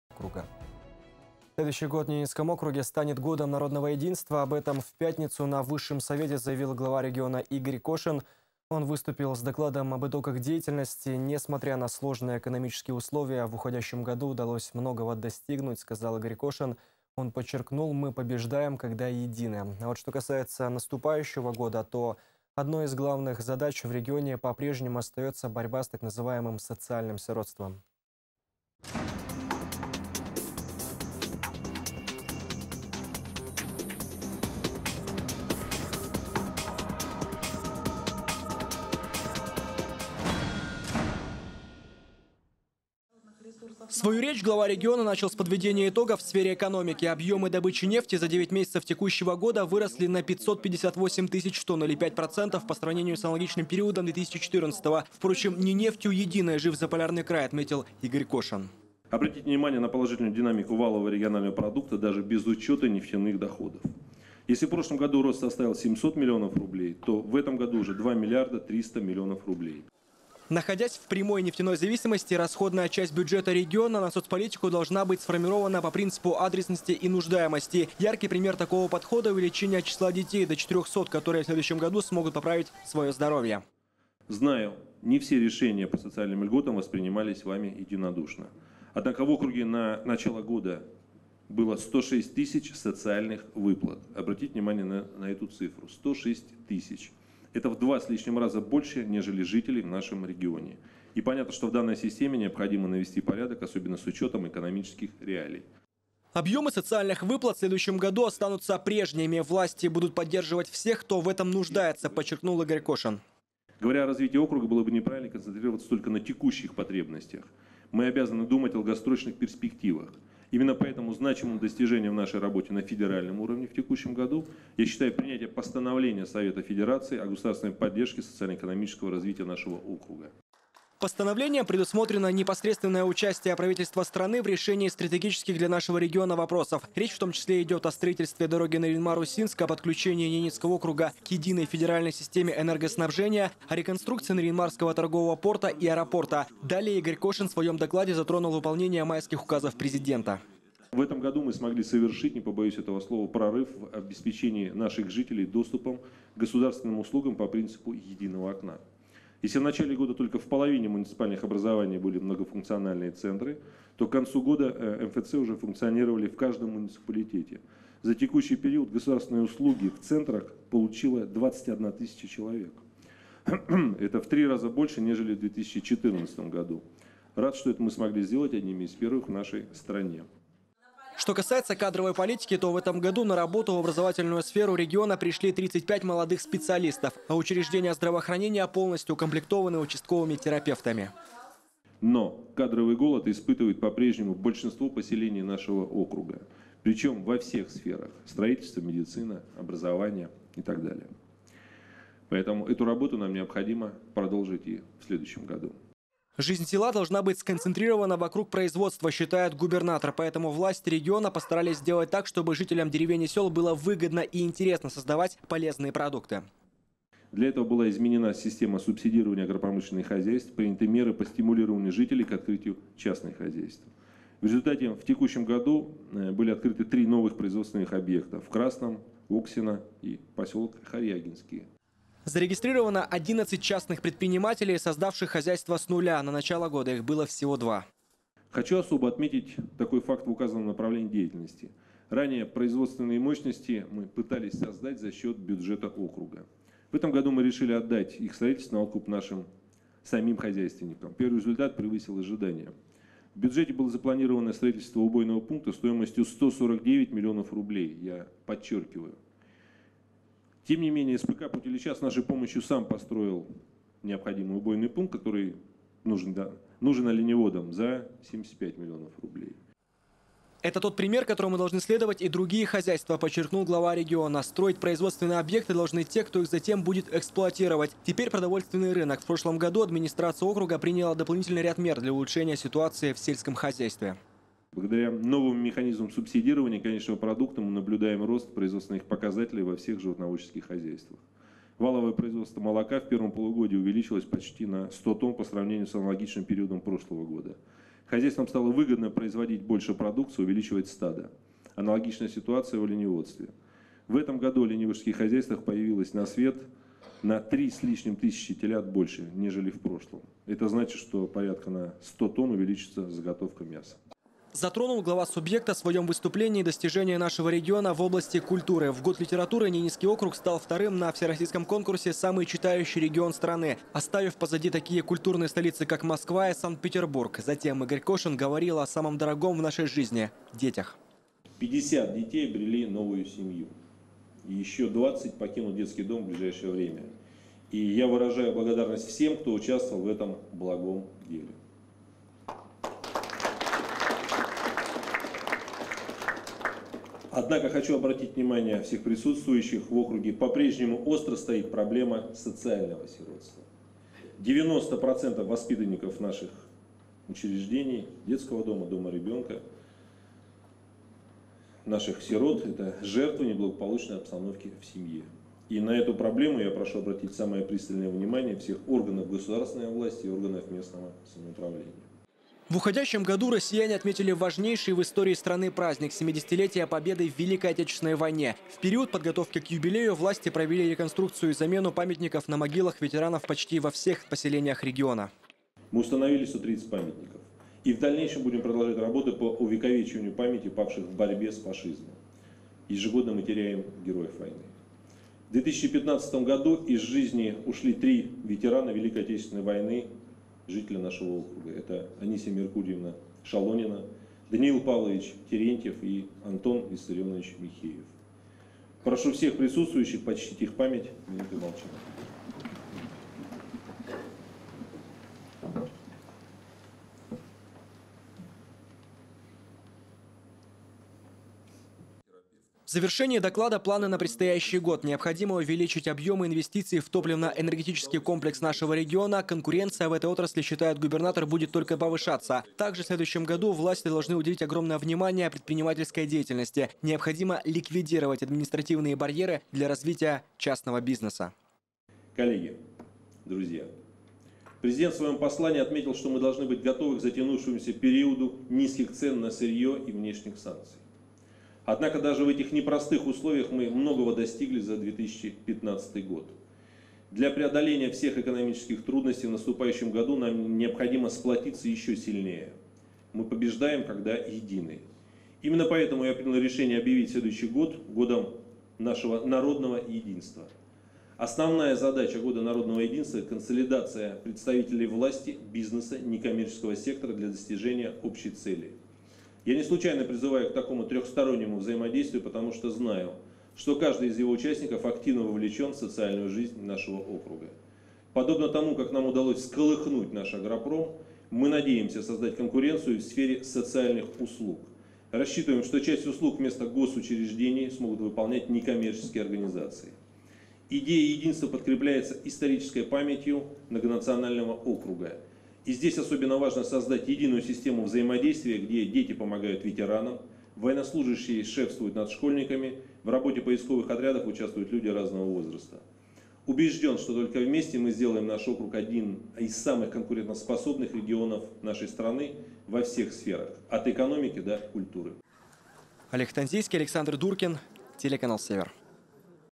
Губернатор НАО выступил с итоговым отчетом за 2015 год
Следующий год в Ненецком округе станет Годом народного единства. Об этом сегодня на высшем совете заявил глава региона Игорь Кошин. Он выступил с докладом об итогах деятельности за год. Несмотря на сложные экономические условия, в уходящем году удалось многого достигнуть, сказал Игорь Кошин. Он подчеркнул: «Мы побеждаем, когда едины».

Свою речь глава региона начал с подведения итогов в сфере экономики. Объемы добычи нефти за 9 месяцев текущего года выросли на 558 тысяч тонн, или 5 процентов по сравнению с аналогичным периодом 2014-го. Впрочем, не нефтью единой жив Заполярный край, отметил Игорь Кошин:

— Обратите внимание на положительную динамику валового регионального продукта даже без учета нефтяных доходов. Если в прошлом году рост составил 700 млн рублей, то в этом — уже 2 млрд 300 млн рублей.

Находясь в прямой нефтяной зависимости расходная часть бюджета региона на соцполитику должна быть сформирована по принципу адресности и нуждаемости. Яркий пример такого подхода — увеличение числа детей, которые в следующем году смогут поправить здоровье. Объемы социальных выплат в следующем году останутся прежними. Власти будут поддерживать всех, кто в этом нуждается, подчеркнул Игорь Кошин.

Постановлением предусмотрено непосредственное участие правительства страны в решении стратегических для нашего региона вопросов. Речь, в том числе, идет о строительстве дороги Нарьян-Мар — Усинск, о подключении Ненецкого округа к единой федеральной системе энергоснабжения, о реконструкции Нарьян-Марского торгового порта и аэропорта.

Также речь в докладе шла о  выполнении майских указов президента, кадровой политике в НАО, жизни села, поддержке бизнеса  и др.